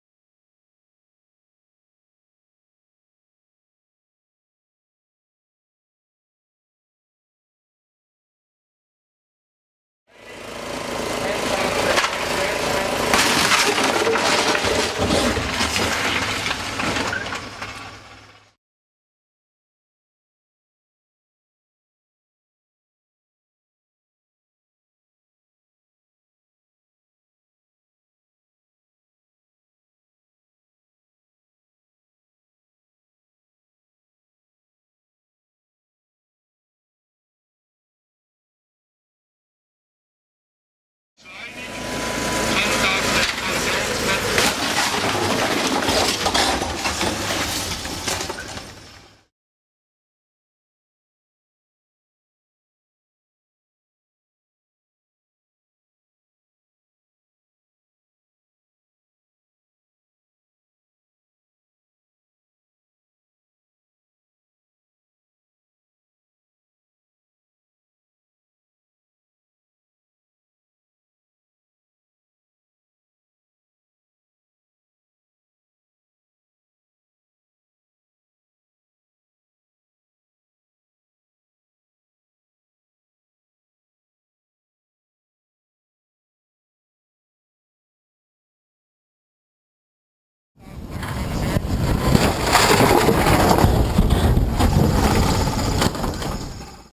Peugeot 306 Vs. Home-Made Wood Hauler Frontal Impact (Rural Roadside Hazard Series)
A home-made ATV is pitted against at Peugeot at a combined speed of nearly 48 Km/h.